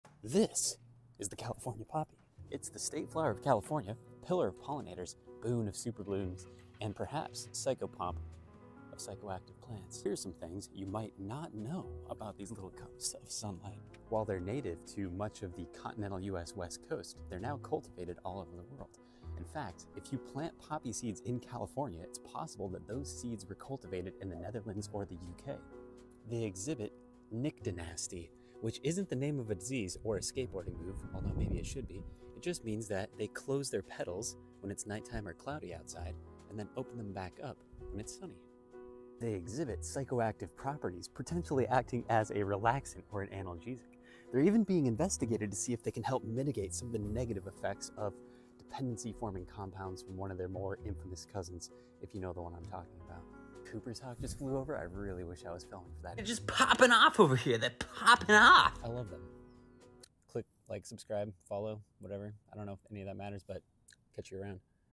They’re poppin off over here
It’s time to get… nyctinasty. Eschscholzia californica, or the California poppy, is one of my favorite spring blooms in California, but you can catch them in many places all over the world now.